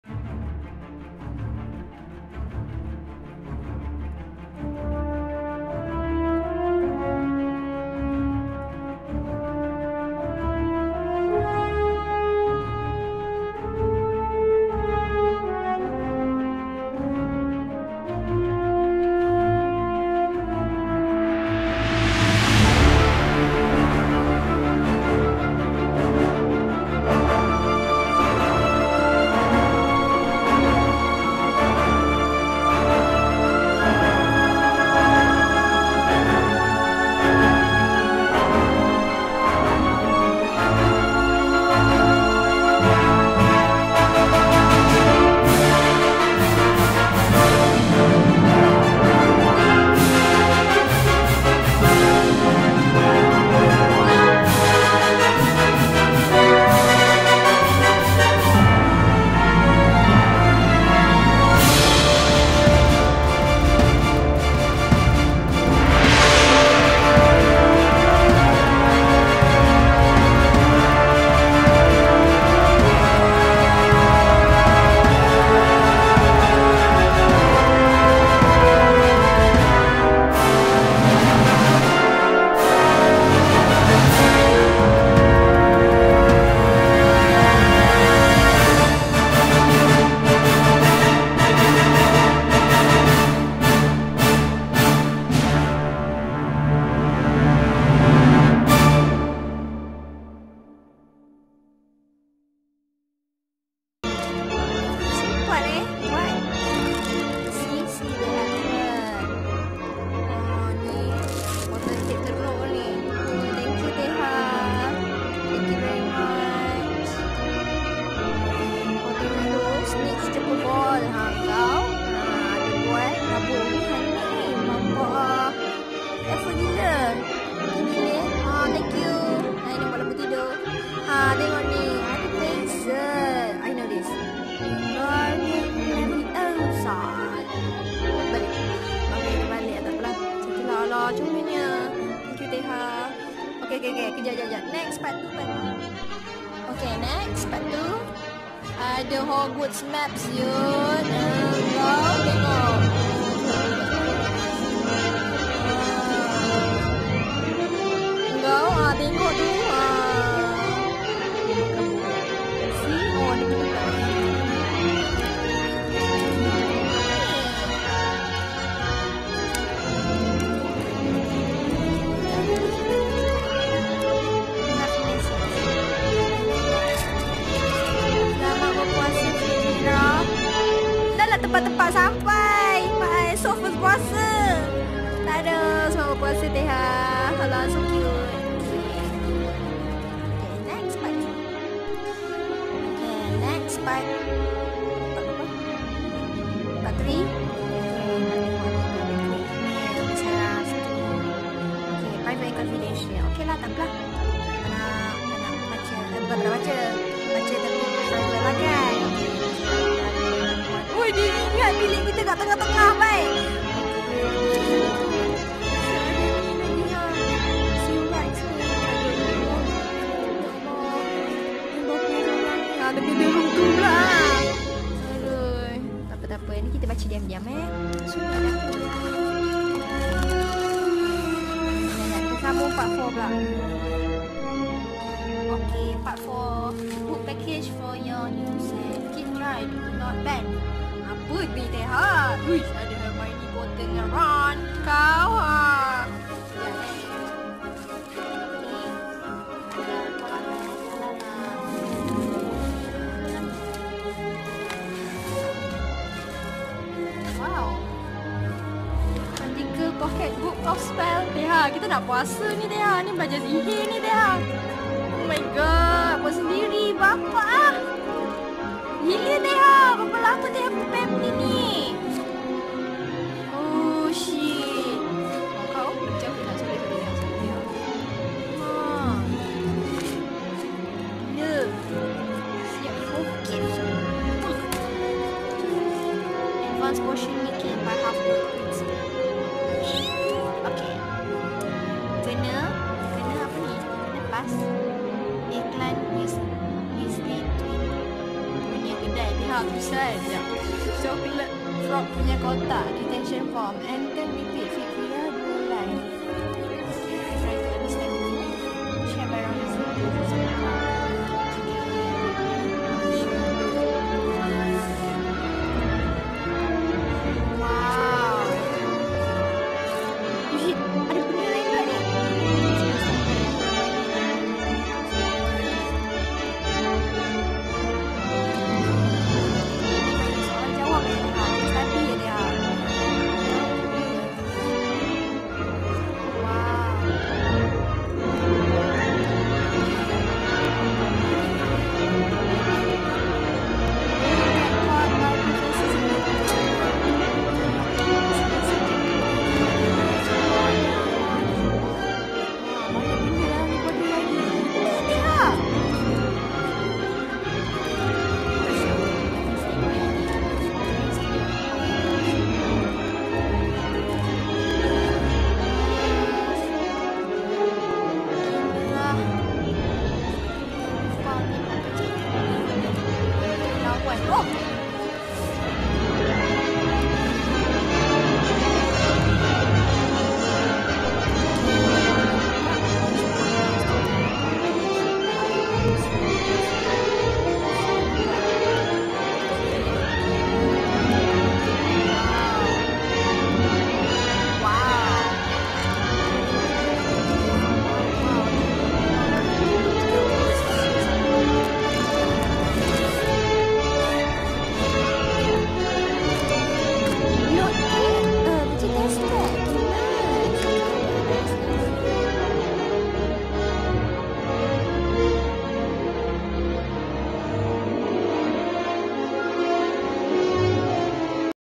UNBOXING Harry Potter theme birthday gift ⚡
I made a HP theme birthday gift to my twin friends✌🏻unboxing video credit to my friends 🥰
Thanks to my fav HP workshop YT , Wizardry Workshop. THANKS SO MUCH ❤